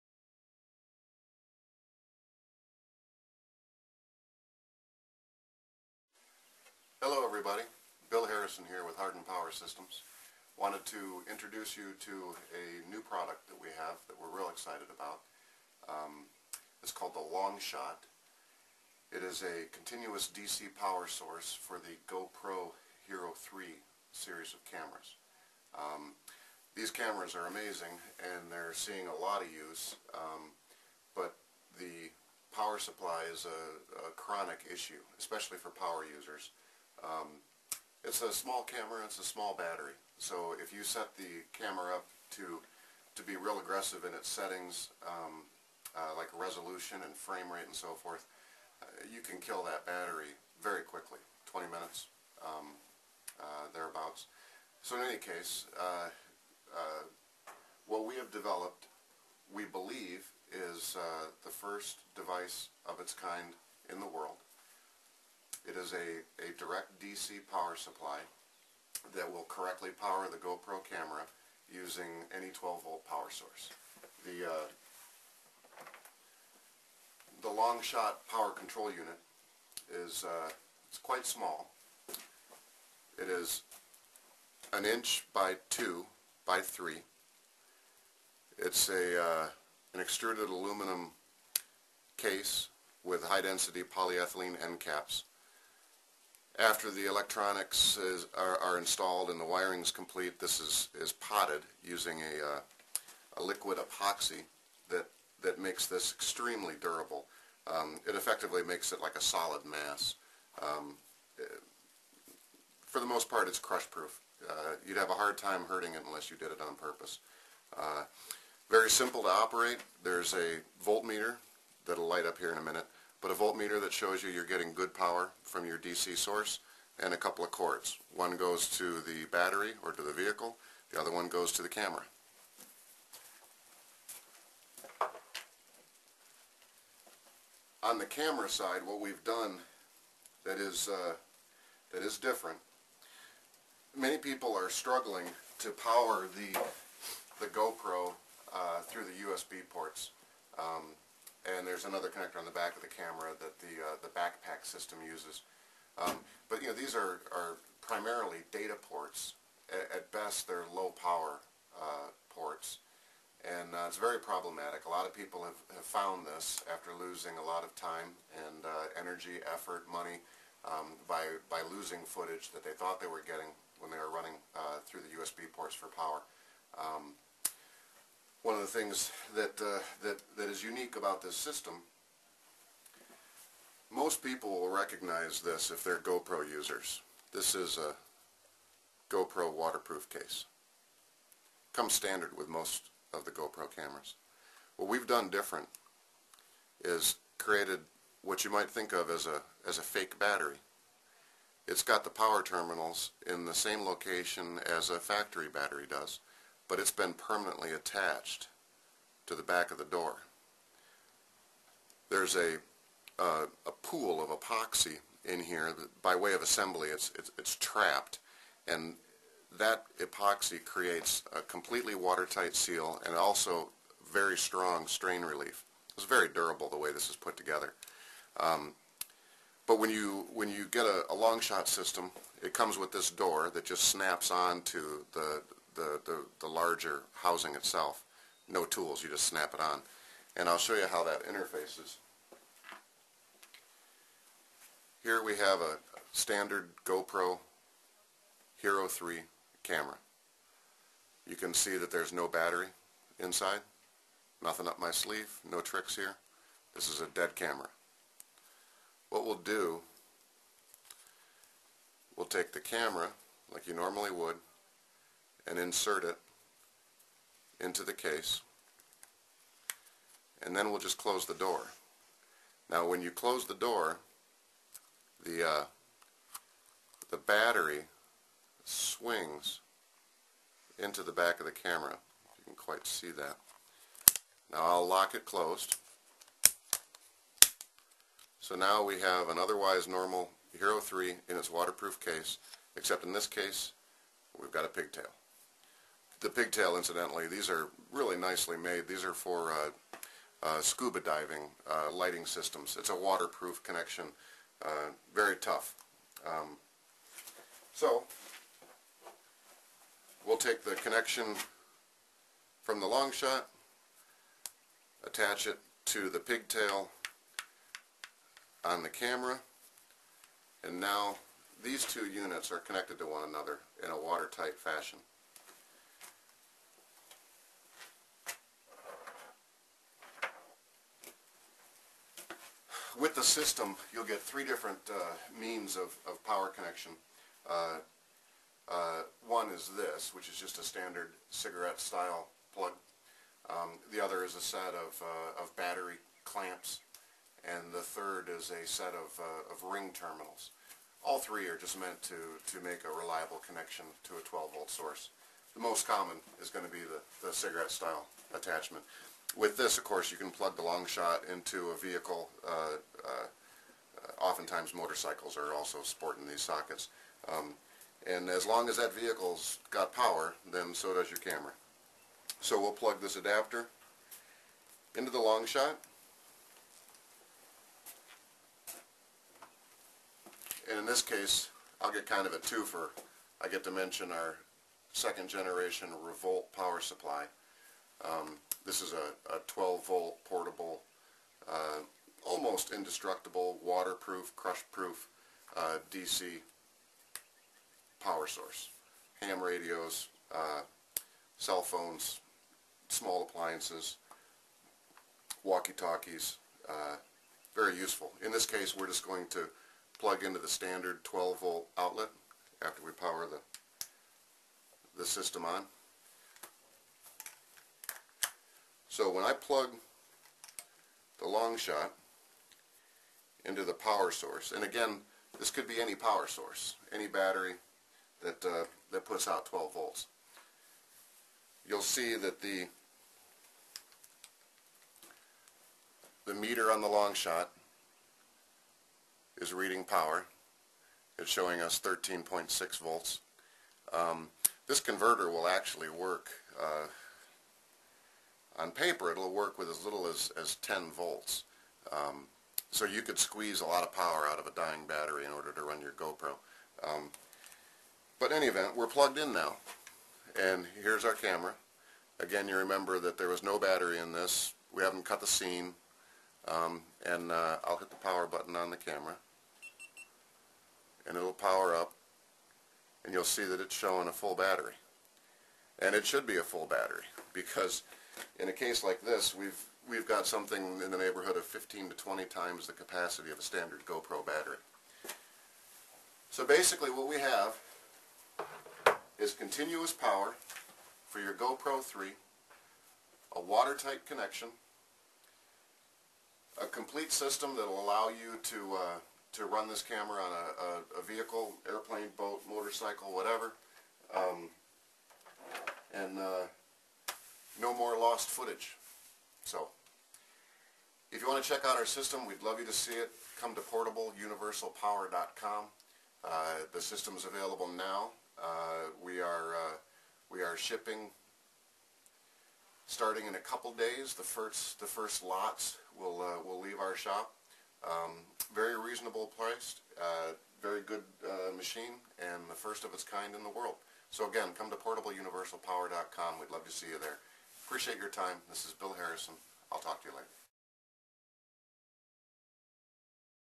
LongShot GoPro Power Supply
From Hardened Power Systems - Another world's first!!  A brief introduction to the LongShot GoPro Power Supply.  Our unique device will power a GoPro indefinitely - as long as you have a 12 volt power source, you can run your GoPro.  Also, the connection to the GoPro is waterproof!   The LongShot will work from a cigarette lighter or directly from a battery.   Visit PortableUniversalPower.com for more info or to purchase.